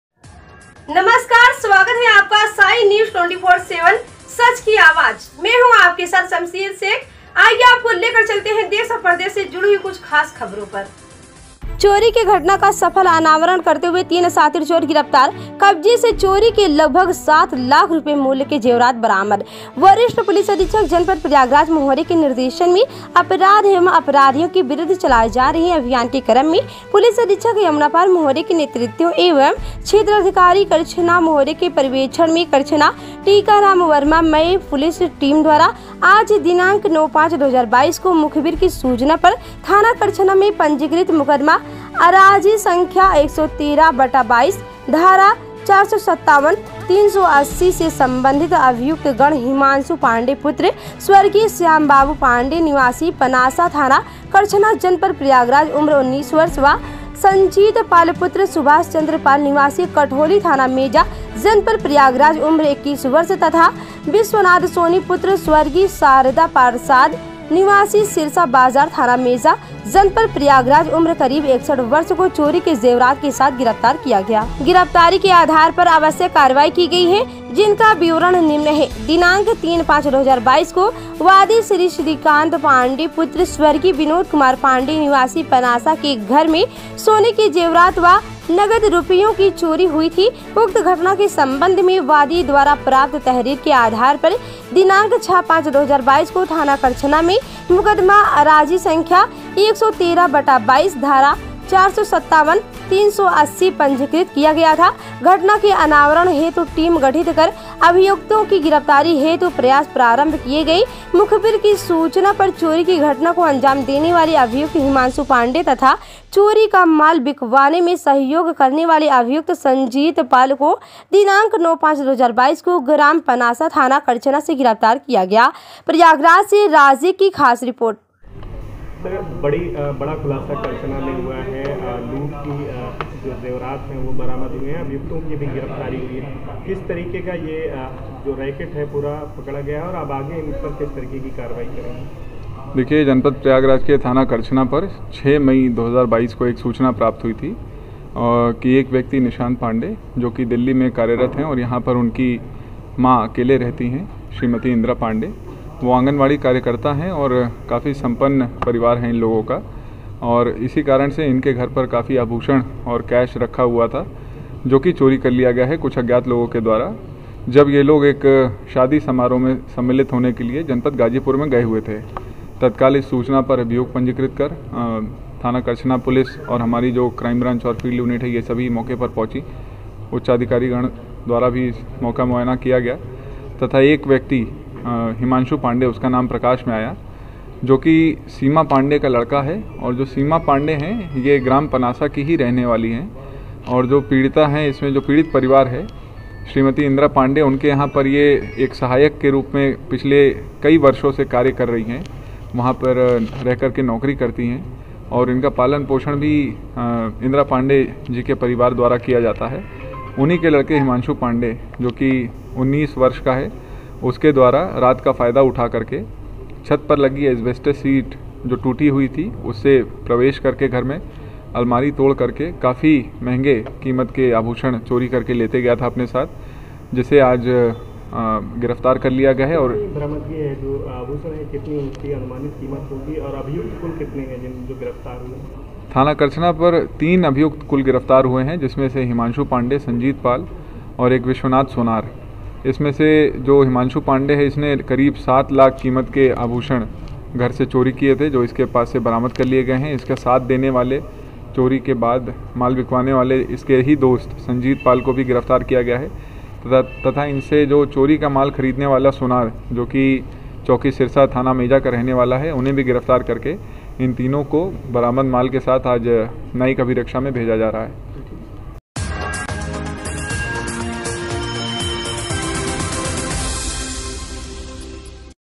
चोरी की घटना का सफल अनावरण करते हुए 03ोर गिरफ्तार, कब्जे से चोरी के लगभग सात लाख रुपये मूल्य के
चोरी की घटना का सफल अनावरण करते हुए 03
शातिर चोर गिरफ्तार, कब्जे से चोरी के लगभग सात लाख रुपये मूल्य के जेवरात बरामद

 वरिष्ठ पुलिस अधीक्षक जनपद प्रयागराज महोदय के निर्देशन मे अपराध एवं अपराधियों के विरुद्ध चलाए जा रहे अभियान के क्रम मे  पुलिस अधीक्षक यमुनापार महोदय के नेतृत्व एवं क्षेत्राधिकारी करछना महोदय के पर्यवेक्षण मे करछना टीकाराम वर्मा मय पुलिस टीम द्वारा आज दिनांक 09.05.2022 को मुखबिर की सूचना पर थाना करछना में पंजीकृत मुकदमा अराजी  संख्या 113/22 धारा 457/380 से सम्बन्धित अभियुक्तगण हिमान्शु पाण्डेय पुत्र स्वर्गीय श्यामबाबू पाण्डेय निवासी पनासा थाना करछना जनपद प्रयागराज उम्र 19 वर्ष, व संजीत पाल पुत्र सुभाषचन्द्र पाल निवासी कठौली थाना मेजा जनपद प्रयागराज उम्र 21 वर्ष तथा विश्वनाथ सोनी पुत्र स्वर्गीय शारदा प्रसाद निवासी सिरसा बाजार थाना मेजा
जनपद प्रयागराज उम्र करीब 61 वर्ष को चोरी के जेवरात के साथ गिरफ्तार किया गया। गिरफ्तारी के आधार पर आवश्यक कार्यवाही की गयी है जिनका विवरण निम्न है-दिनांक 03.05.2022 को वादी श्री निशान्त पाण्डेय पुत्र स्वर्गीय विनोद कुमार पाण्डेय निवासी पनासा के घर में सोने के जेवरात व नगद रूपयों की चोरी हुयी थी । उक्त घटना के सम्बन्ध में वादी द्वारा प्राप्त तहरीर के आधार
पर दिनांक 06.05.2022 को थाना करछना में मुकदमा अराजी संख्या 113/22 धारा 457/380 पंजीकृत किया गया था।घटना के अनावरण हेतु टीम गठित कर अभियुक्तों की गिरफ्तारी हेतु प्रयास प्रारम्भ किए गये। मुखबिर की सूचना पर चोरी की घटना को अंजाम देने वाले अभियुक्त हिमान्शु पाण्डेय तथा चोरी का माल बिकवाने में सहयोग करने वाले अभियुक्त संजीत पाल को दिनांक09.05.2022 को ग्राम पनासा थाना करछना से गिरफ्तार

प्रयागराज से राजीक की रिपोर्ट